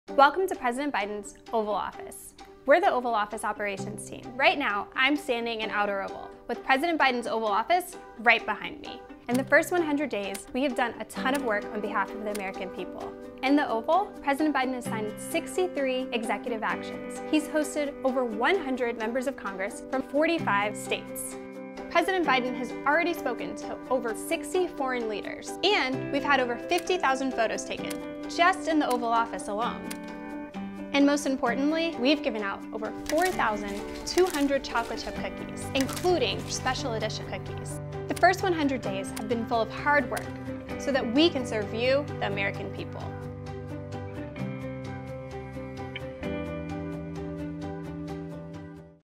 President Biden’s Oval Office 100 Days By The Numbers
From 63 executive actions signed to 4,200 chocolate chip cookies given out — here’s a by the numbers look at President Biden’s first 100 days in the Oval Office.